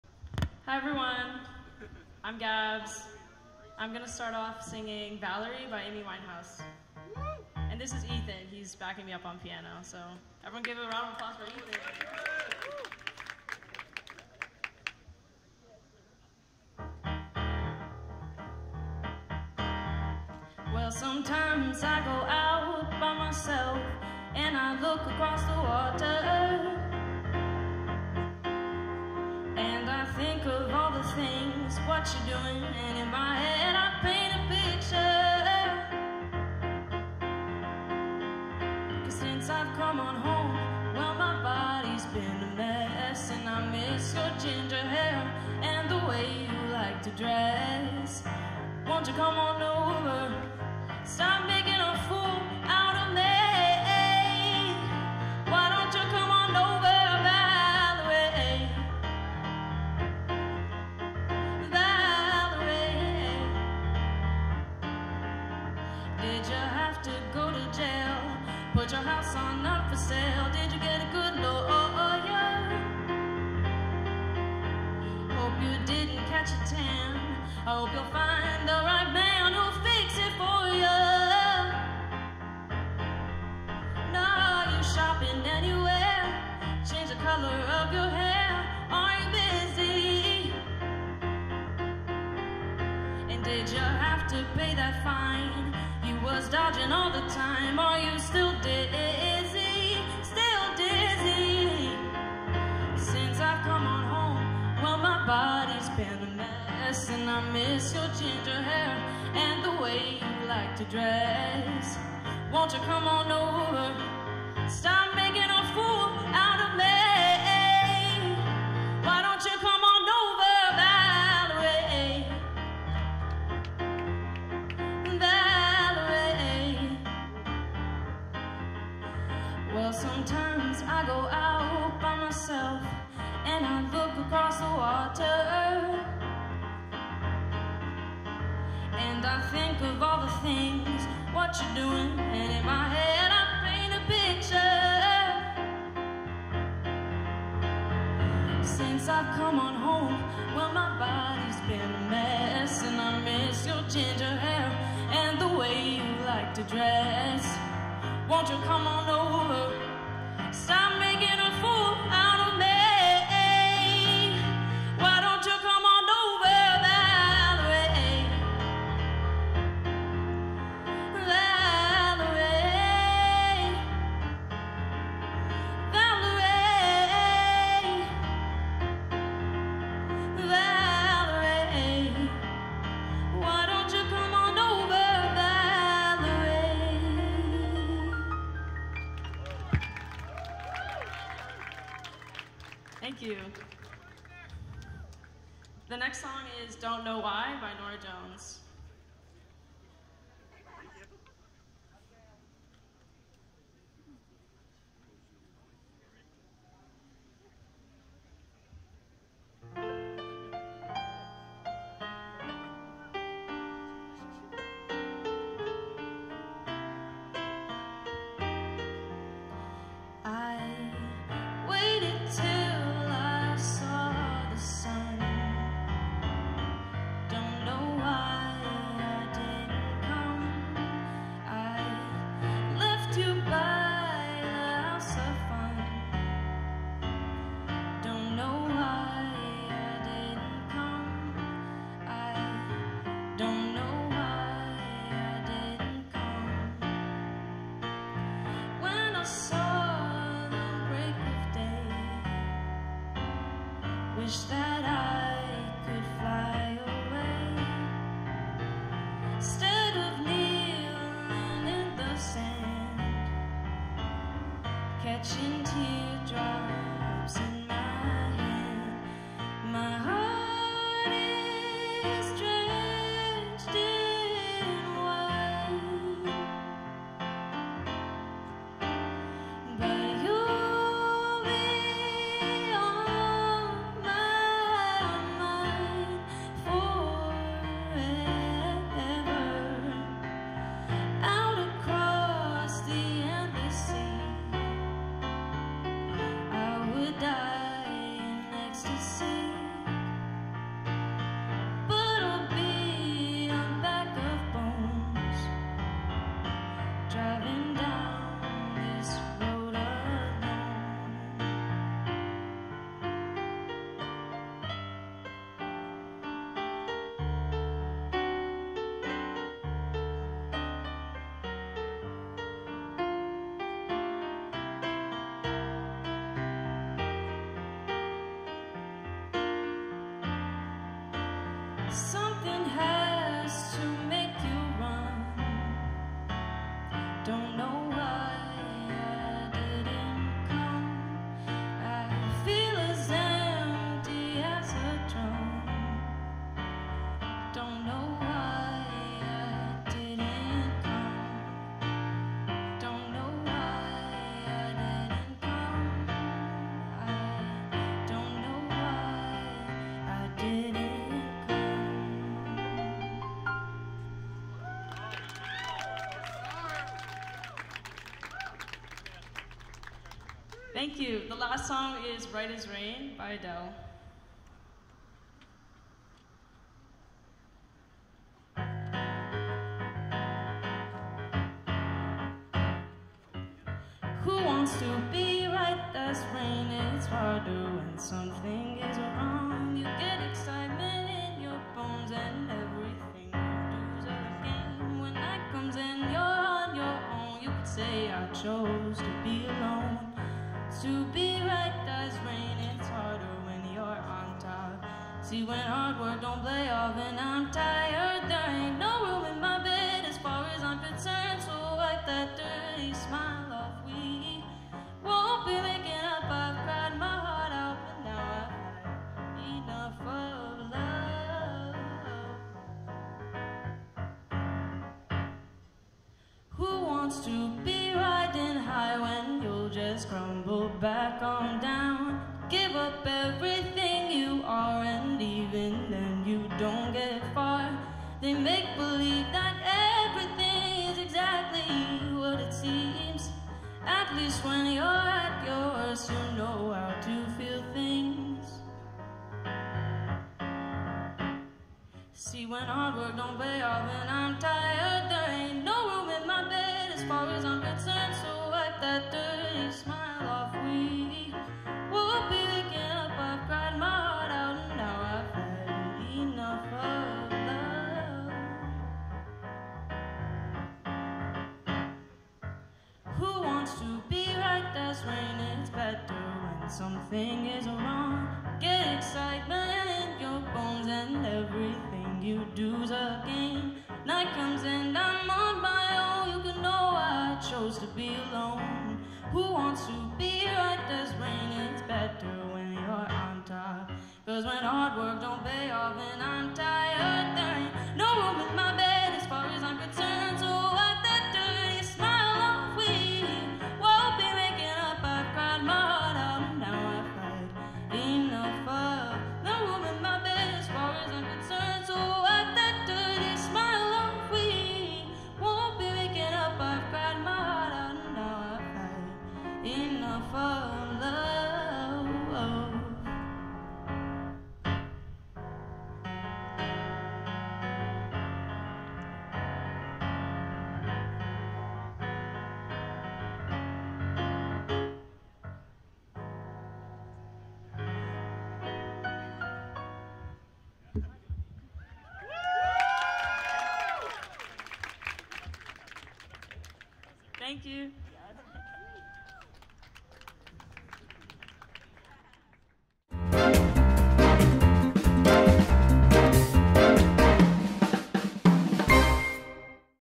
Gabriella Capizzi - 6/5/21 Variety Show
Gabriella Capizzi's set from The Hall CP x Rails Comedy Variety Show on June 5, 2021. .